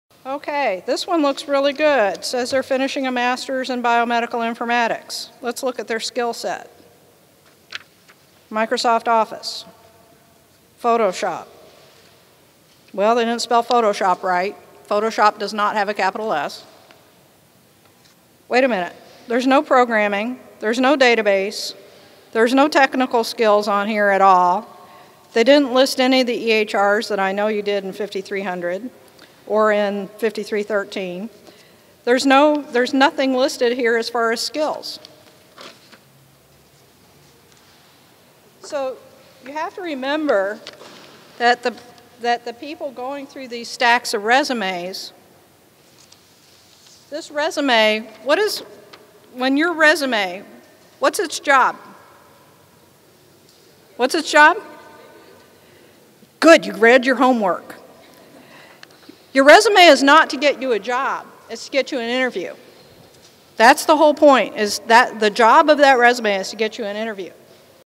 Informatics Resume 101 - Informatics Skills & Purpose of the Resume
Continuing with our "Informatics Resume 101" series Dr. Smith tells us what Informatics skills to list and what the purpose of a resume is.